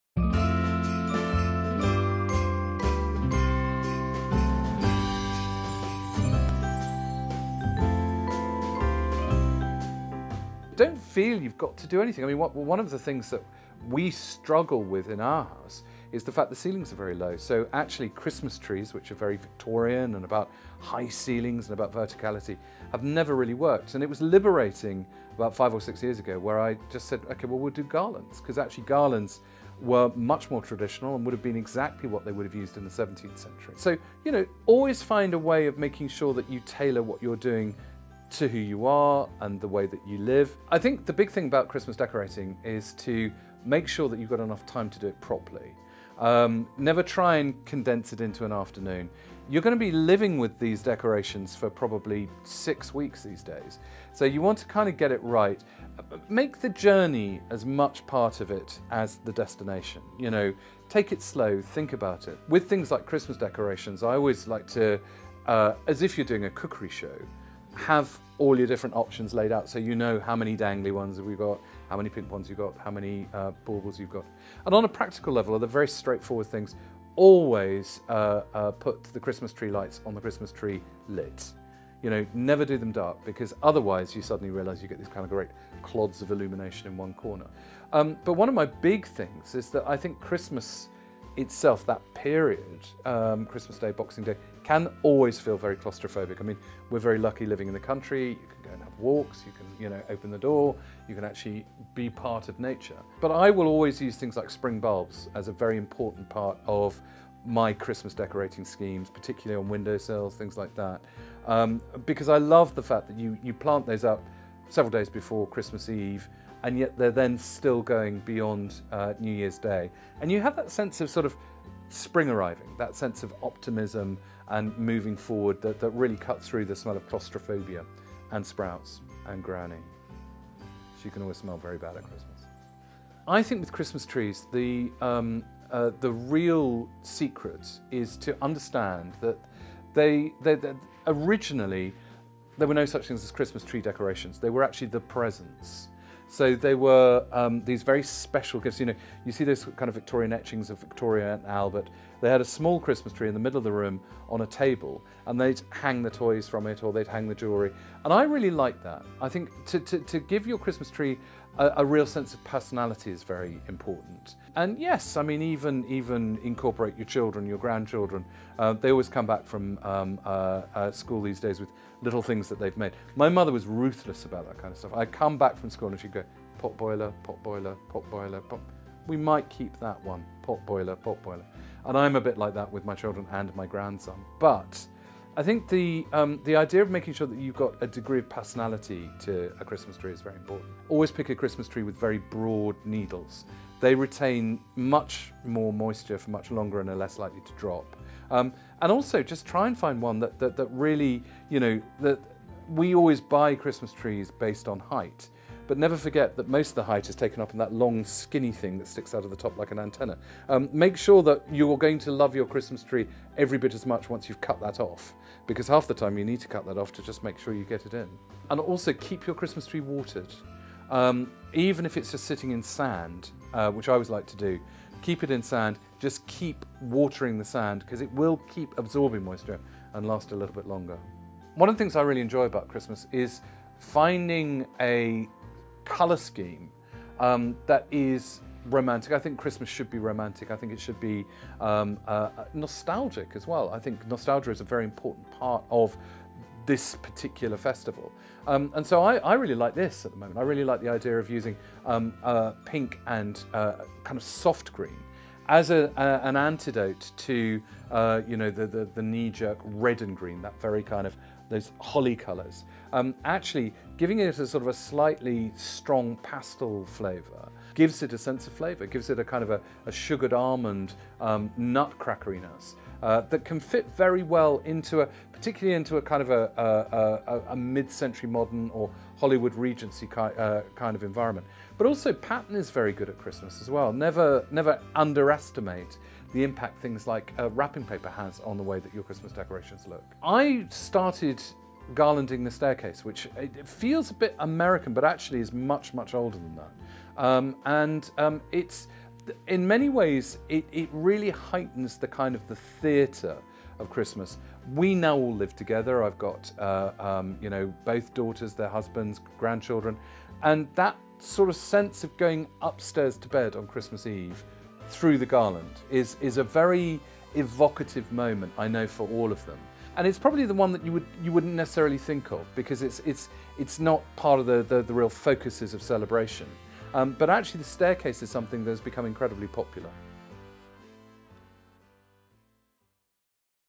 Laurence Llewelyn-Bowen's 9 Christmas Decorating Tips
9 Christmas decorating lessons from interior design legend Laurence Llewelyn-Bowen.